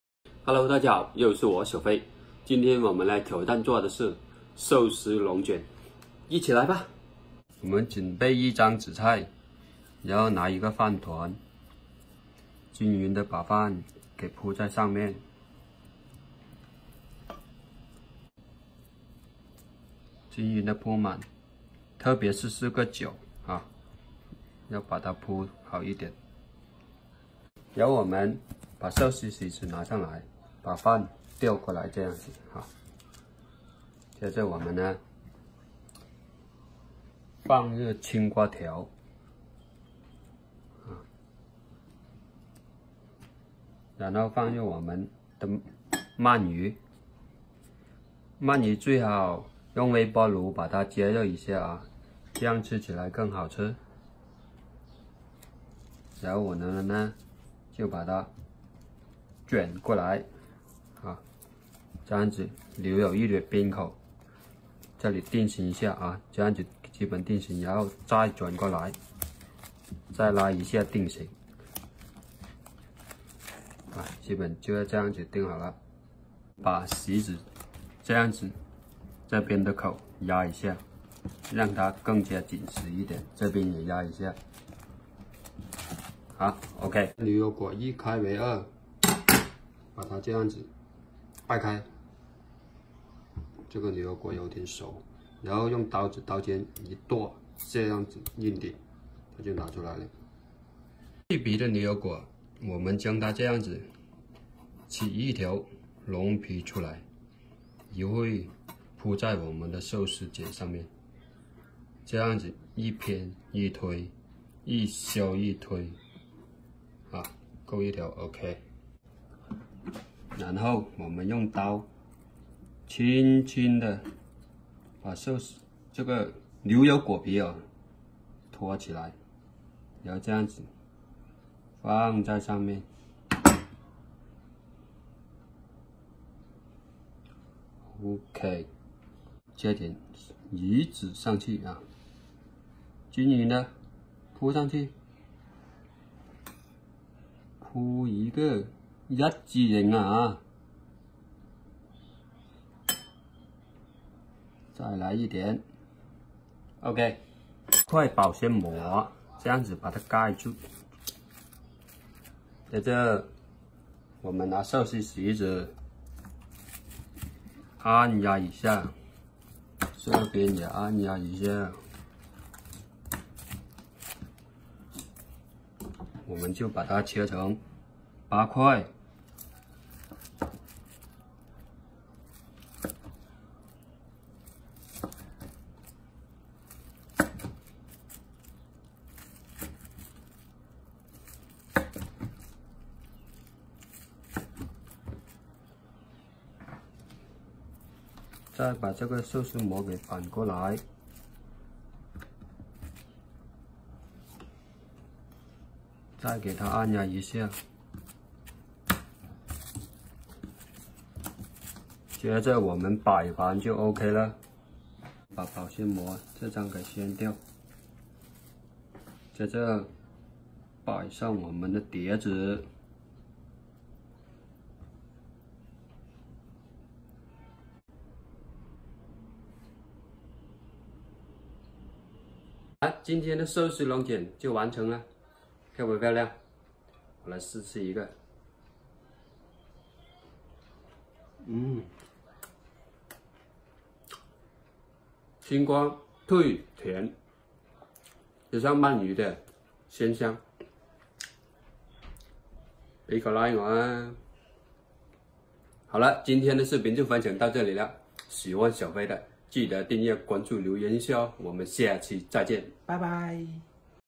(牛油果)如何制作寿司龙卷？How to make sushi rolls 1.0版本【沉迷美食小飞】
(牛油果)如何制作寿司龙卷，好吃又不上火，希望你们喜欢本期影片。
本期影片需要的食材：牛油果，鳗鱼，青瓜，鱼籽，醋饭，紫菜。